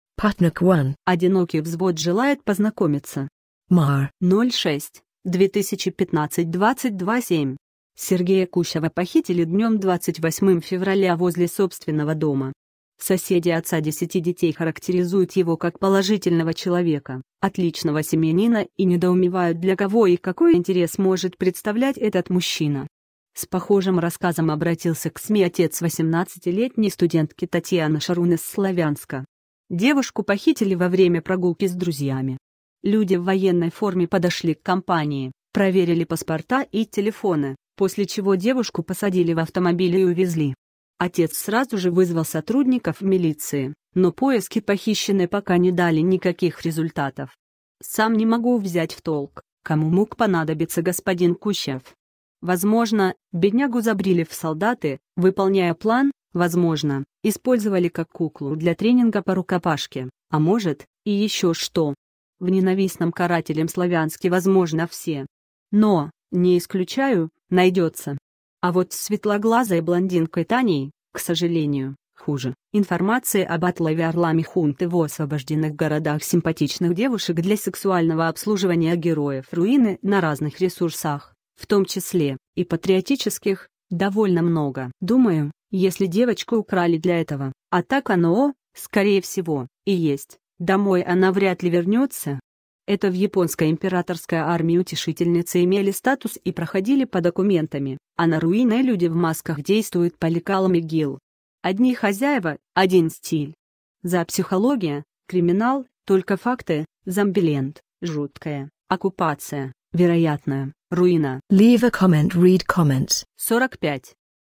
LJ150306 037 putnik1 ОДИНОКИЙ ВЗВОД ЖЕЛАЕТ ПОЗНАКОМИТЬСЯ
putnik1 ОДИНОКИЙ ВЗВОД ЖЕЛАЕТ ПОЗНАКОМИТЬСЯ Mar 06, 2015 22:07 Сергея Кущова похитили днём 28 февраля возле собственного дома. Соседи отца десяти детей характеризуют его как положительного человека, отличного семьянина и недоумевают для кого и какой интерес может представлять этот мужчина... С похожим рассказом обратился к СМИ отец 18-летней студентки Татьяны Шарун из Славянска. Девушку похитили во время прогулки с друзьями. Люди в военной форме подошли к компании, проверили паспорта и телефоны, после чего девушку посадили в автомобиль и увезли. Отец сразу же вызвал сотрудников милиции, но поиски похищенной пока не дали никаких результатов... Сам не могу взять в толк, кому мог понадобиться г-н Кущов. Возможно, беднягу забрили в солдаты, выполняя план, возможно, использовали как куклу для тренинга по рукопашке, а может, и еще что. В ненавистном карателям Славянске возможно все. Но, не исключаю, найдется. А вот с светлоглазой блондинкой Таней, к сожалению, хуже: информации об отлове орлами хунты в "освобожденных городах" симпатичных девушек для сексуального обслуживания героев Руины на разных ресурсах (в том числе, и "патриотических) довольно много. Думаю, если девочку украли для этого, - а так оно, скорее всего, и есть, - домой она вряд ли вернется. Это в японской императорской армии "утешительницы" имели статус и проходили по документами, а на Руине "люди в масках" действуют по лекалам ИГИЛ. Одни хозяева, один стиль. зоопсихология, криминал, только факты, зомбиленд, жуткое, оккупация, вероятное, руина Leave a comment Read comments 45